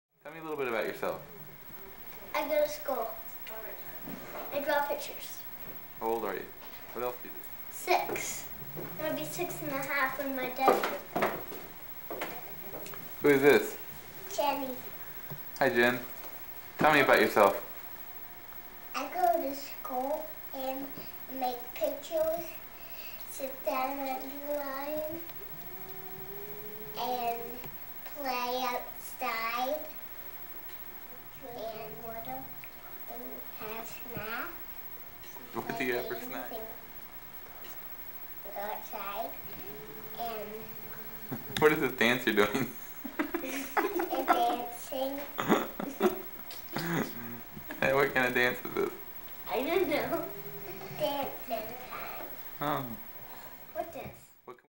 Girardi Kids - Dancing
Jimmy and Jenny Answering Questions and dancing